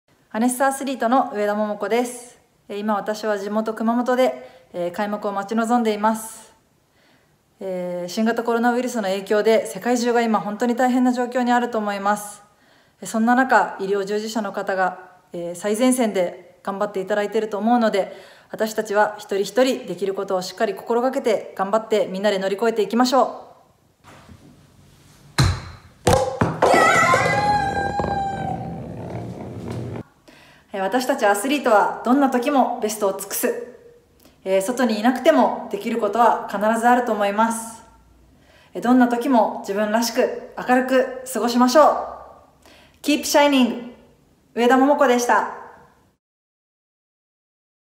上田桃子選手、紙コップに“ホールインワン” 「みんなでがんばろー」応援メッセージ届ける アネッサ「KeepShining」プロジェクト
資生堂の日焼け止めブランド「アネッサ」は、「KeepShining」を合言葉に、外出自粛が続く中、自宅ですこやかに過ごしていただくことを目的とする「KeepShining」プロジェクトを、4月30日より始動。

「KeepShining」を合言葉にリレー形式で展開していく動画の、髙梨沙羅選手（4月30日公開）、伊藤ふたば選手（3日公開）に続く、3人目のアスリートとして、プロゴルファーの上田桃子選手の動画が、6日に公開。

　動画は地元熊本の自宅から、最前線で働く医療従事者に向けた応援メッセージと、アプローチショットにチャレンジしたもの。カップの中から現れる、上田選手手作りの「みんなでがんばろー！！」の旗を持ったかわいい人形に注目。